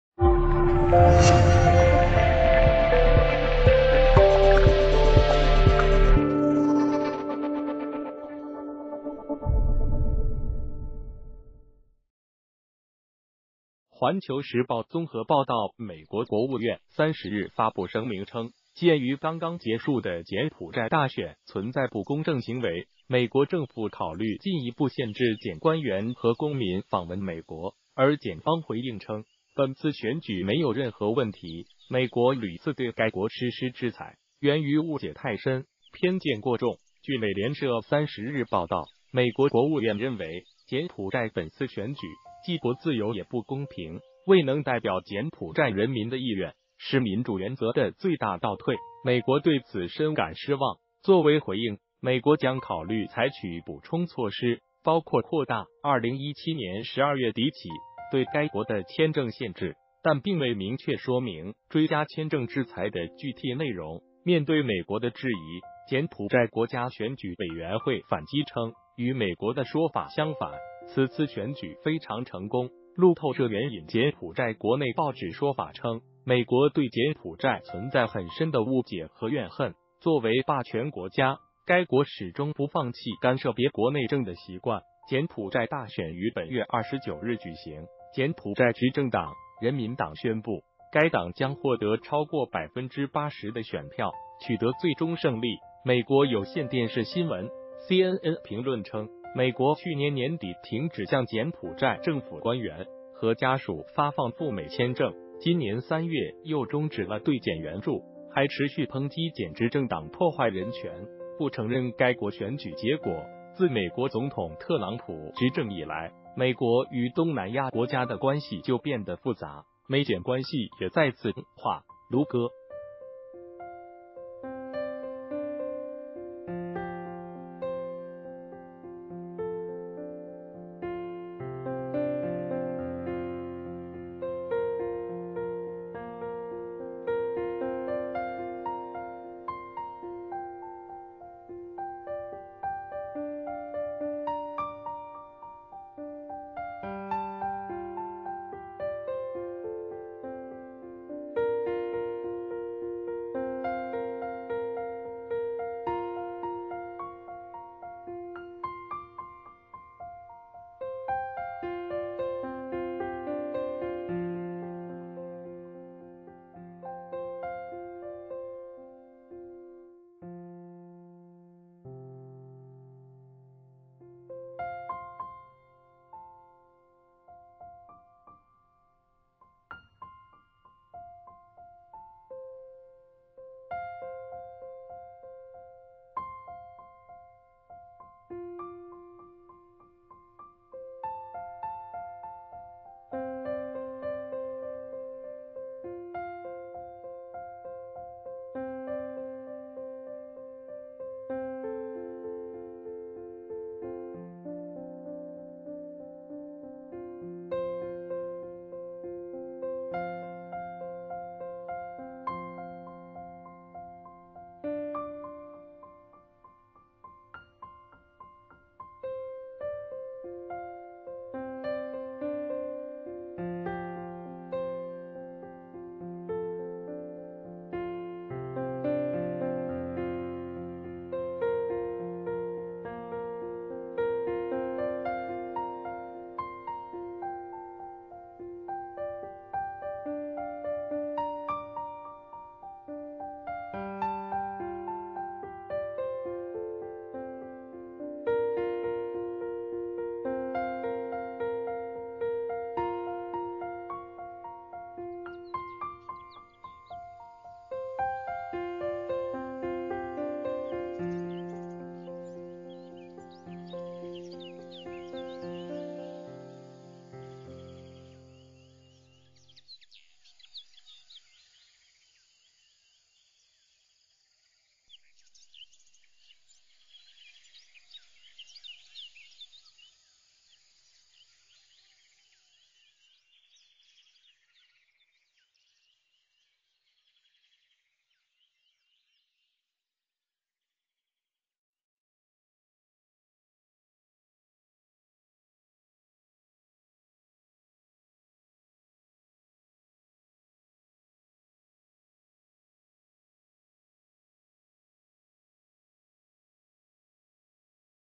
宣称,选举,不公,扬言,扩大,柬埔寨,制裁,方回
谢谢！[环球时报综合报道]美国国务院30日发布声明称，鉴于刚刚结束的柬埔寨大选存在不公正行为，美国政府考虑进一步限制柬官员和公民访问美国。而柬方回应称，本次选举没有任何问题。美国屡次对该国实施制裁，源于“误解太深”，偏见过重。据美联社30日报道，美国国务院认为，柬埔寨本次选举既不自由也不公平，未能代表柬埔寨人民的意愿，是民主原则的最大倒退，美国对此“深感失望”。作为回应，美国将考虑采取补充措施，包括扩大2017年12月底起对该国的签证限制。但并未明确说明追加签证制裁的具体内容。面对美国的质疑，柬埔寨国家选举委员会反击称，与美国的说法相反，此次选举“非常成功”。路透社援引柬埔寨国内报纸说法称，美国对柬埔寨存在很深的误解和怨恨，作为霸权国家，该国始终不放弃干涉别国内政的习惯。柬埔寨大选于本月29日举行，柬埔寨执政党、人民党宣布，该党将获得超过80%的选票，取得最终胜利。美国有线电视新闻(CNN)评论称，美国去年年底停止向柬埔寨政府官员和家属发放赴美签证，今年3月又终止了对柬援助，还持续抨击柬执政党“破坏人权”，不承认该国选举结果。自美国总统特朗普执政以来，美国与东南亚国家的关系就“变得复杂”，美柬关系也再次恶化。(卢戈)